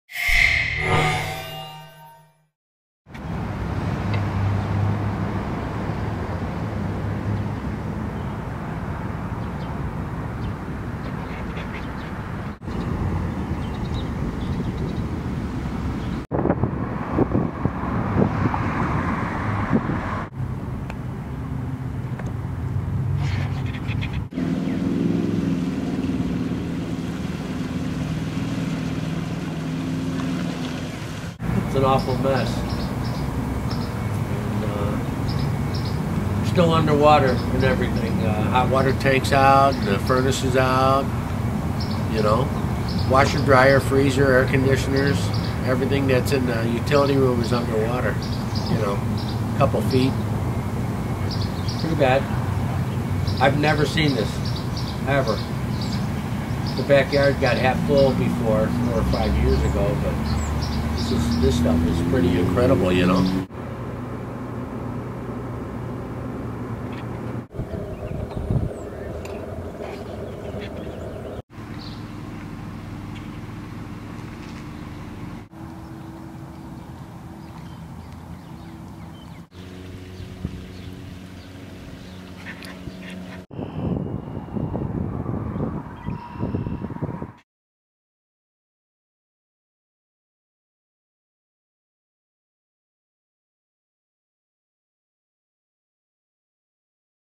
Flooding in Fox Lake
There is widespread flooding around Fox Lake on Monday. The Chain O'Lakes waters have still not receded and area residents are concerned about the forecast of rain on Tuesday.